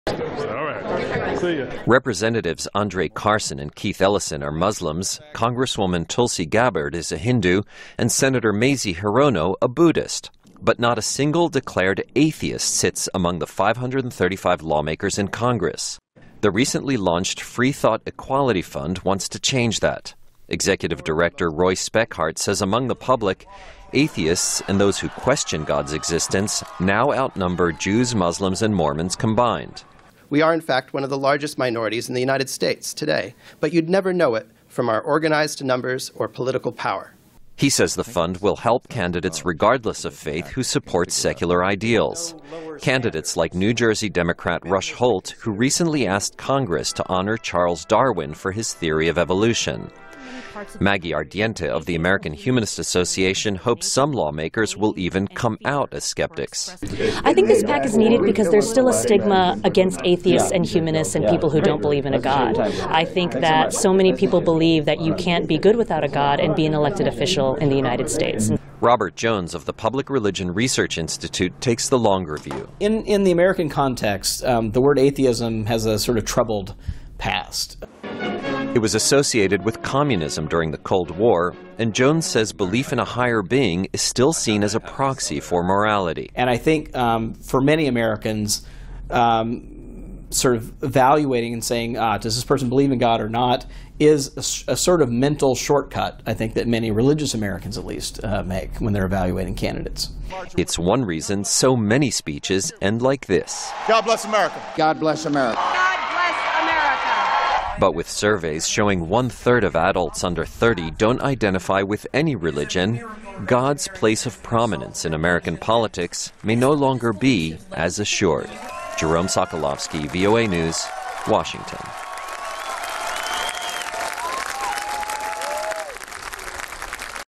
Non-believers Create Political Pressure Group
Polls show that at least six percent of Americans do not believe in God, but their numbers are not reflected in Congress. A new political action committee or PAC will try to win them political representation.